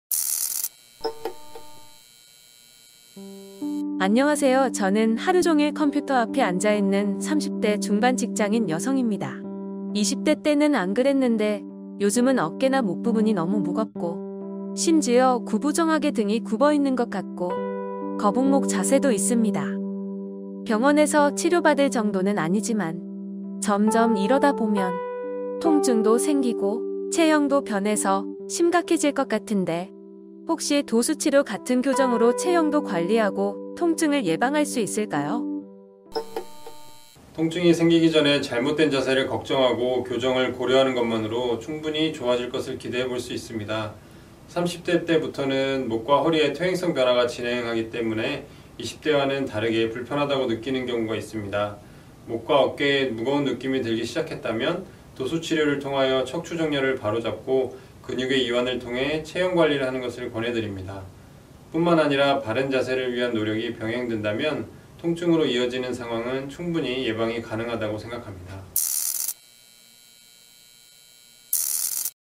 하루종일 컴퓨터앞에 앉아있어서 구부정한 자세가 고민이라면???
안녕하세요 척추 - 관절 - 새움병원입니다!
저는 하루종일 컴퓨터 앞에 앉아있는
30대 중반 직장인 여성입니다.
20대 때는 안그랬는데 요즘은 어깨나 목부분이
너무 무겁고 심지어 구부정하게 등이 굽어있는것 같아요!
거북목 자세도 있답니다...
병원에서 치료받을 정도는 아닌듯 한데
혹시 도수치료 같은 교정으로 체형도 관리하고
통증을 예방할 수 있을까요??

새움병원 척추클리닉 김동윤원장